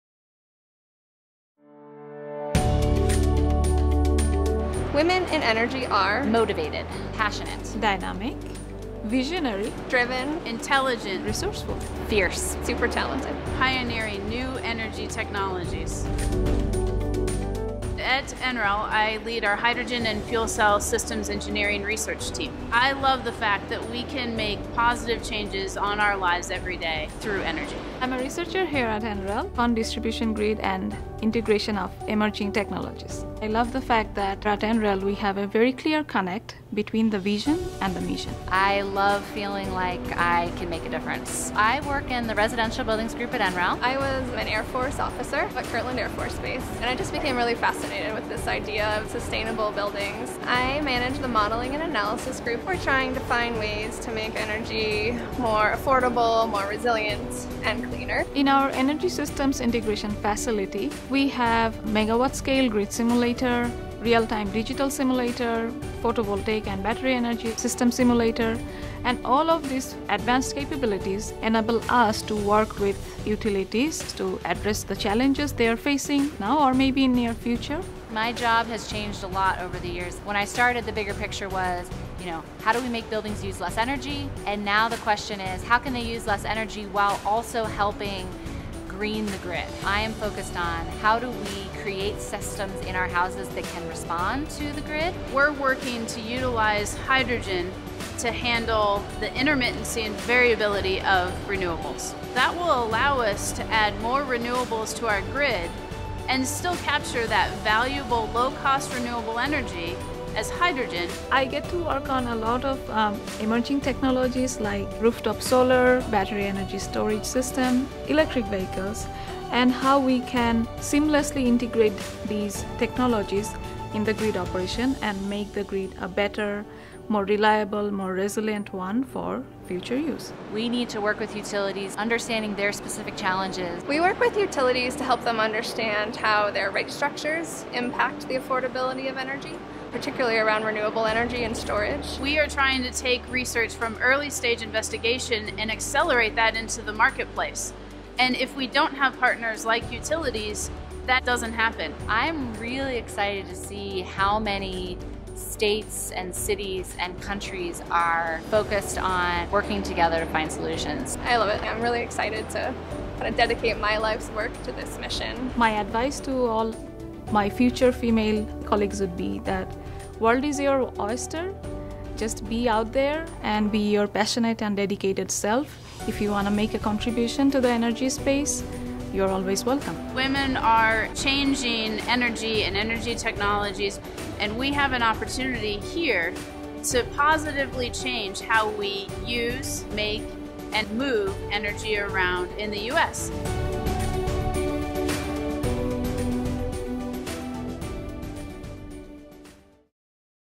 Women in Energy
The women at the lab are researchers, analysts, scientists, engineers, and, most importantly, passionate leaders working for a brighter future who are driven every day to positively impact how we make, store, move and consume energy.

In this short video, learn about some of these talented and dedicated women who are TransformingEnergy